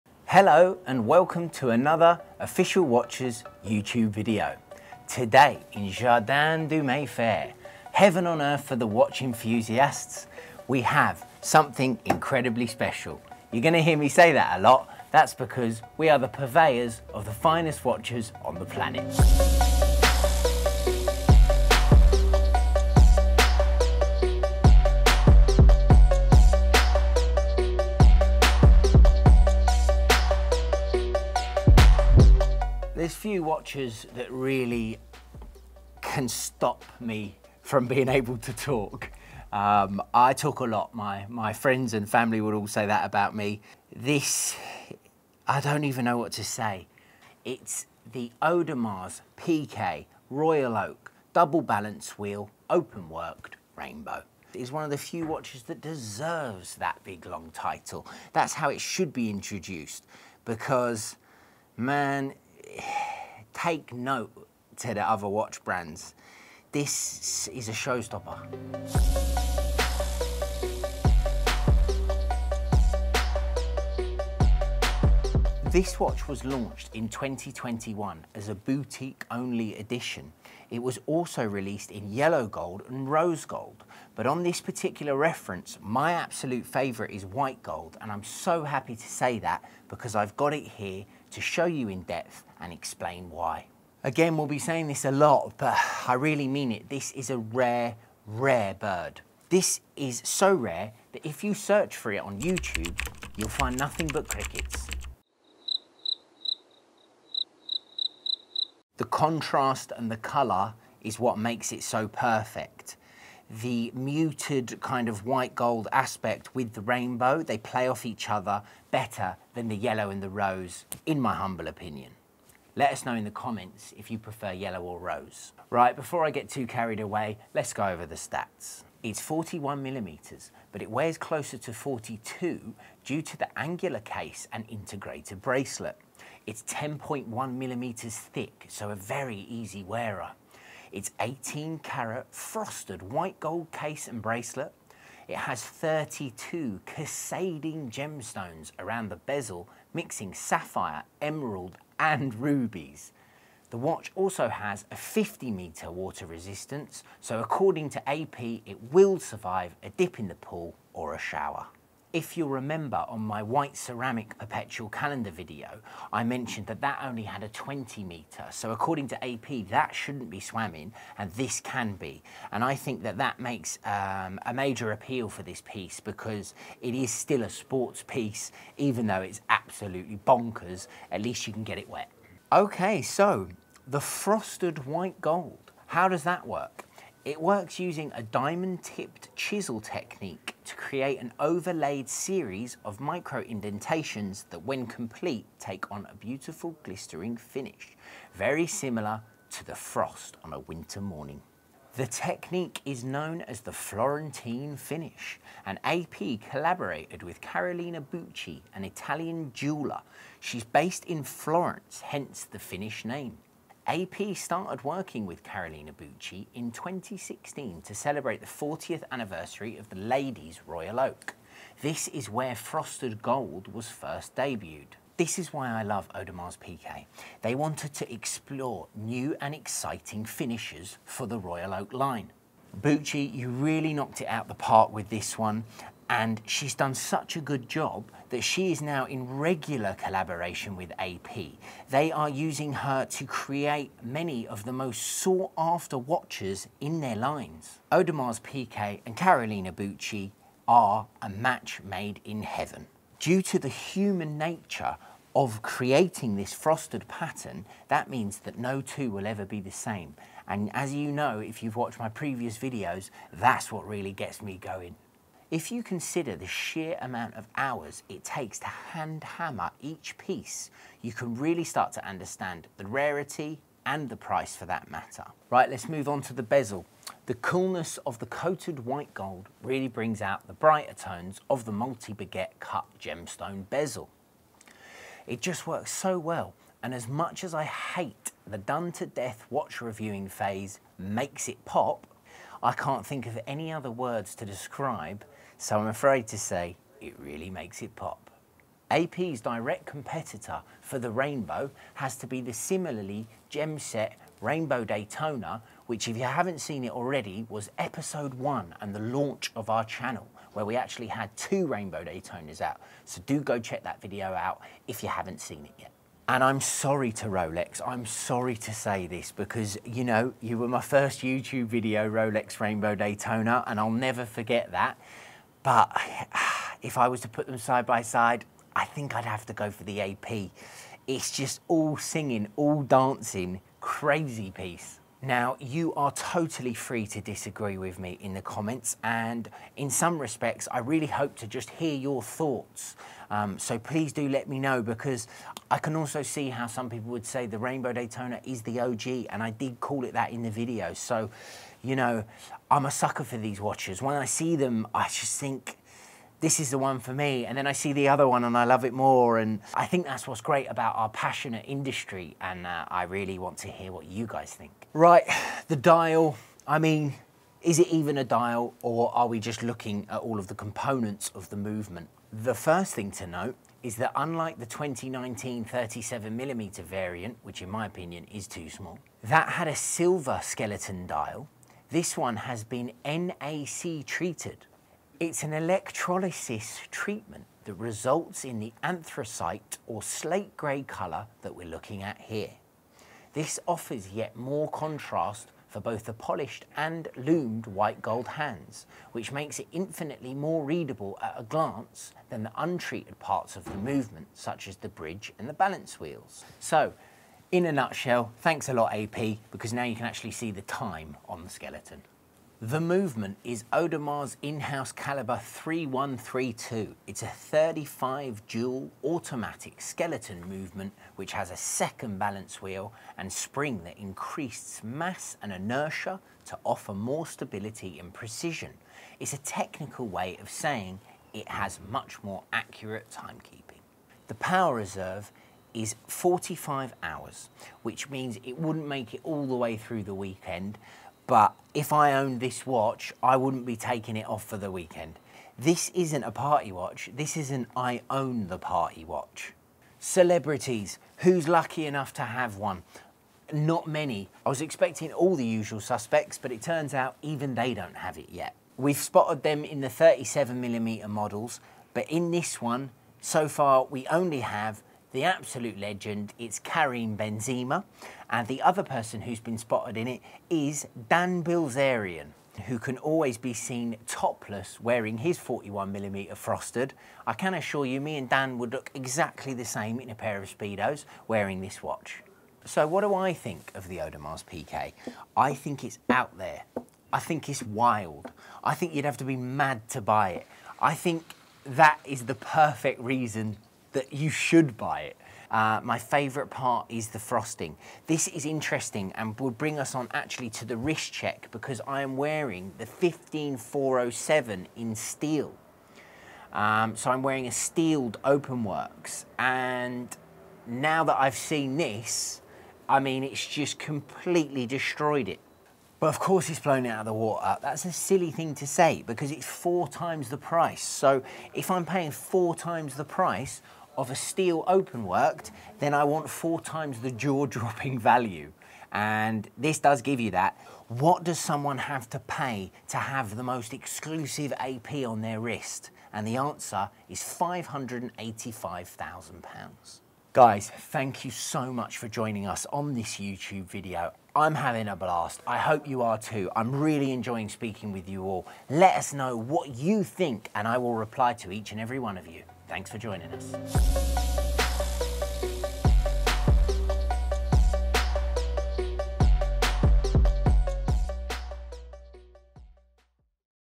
Unveiling the MYTHICAL Frosted AP Royal Oak Rainbow Openworked | 15412BC - Official Watches
Get ready to be transported into a realm of unparalleled elegance and opulence with the Frosted Audemars Piguet Royal Oak Rainbow Openworked. This masterpiece, launched in 2021 as a boutique-exclusive edition, is a rare treasure in the world of haute horlogerie. Crafted from 18-carat frosted white gold and adorned with 32 mesmerizing gemstones, including sapphires, emeralds, and rubies, this watch is a breathtaking symphony of colors and contrasts. Each piece, meticulously crafted using the diamond-tipped chisel technique, is a unique work of art, ensuring exclusivity and individuality. Join us as we explore the mesmerizing world of the Frosted Audemars Piguet Royal Oak Rainbow Openworked and uncover why it stands as a pinnacle of luxury timepieces. Prepare to be captivated like never before.

Oscar is dressed by our dear friends at Hawes & Curtis